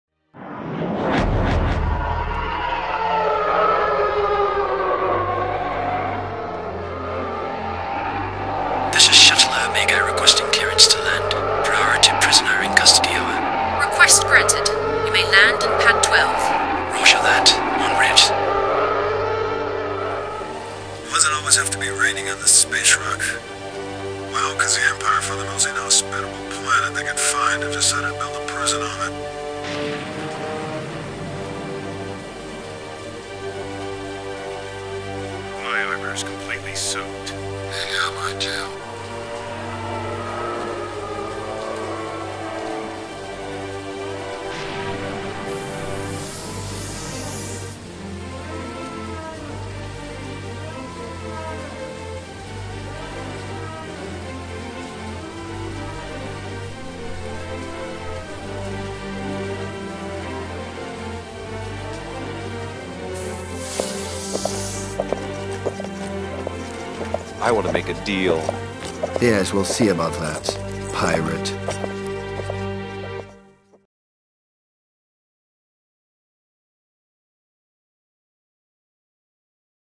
Imperial Prison Transfer
This Cutscene was made for ilios' Embers of the Empire Project, and showcases new capabilities available to Imperial Cinematic, for future Films.

A notorious Pirate has been captured by the empire!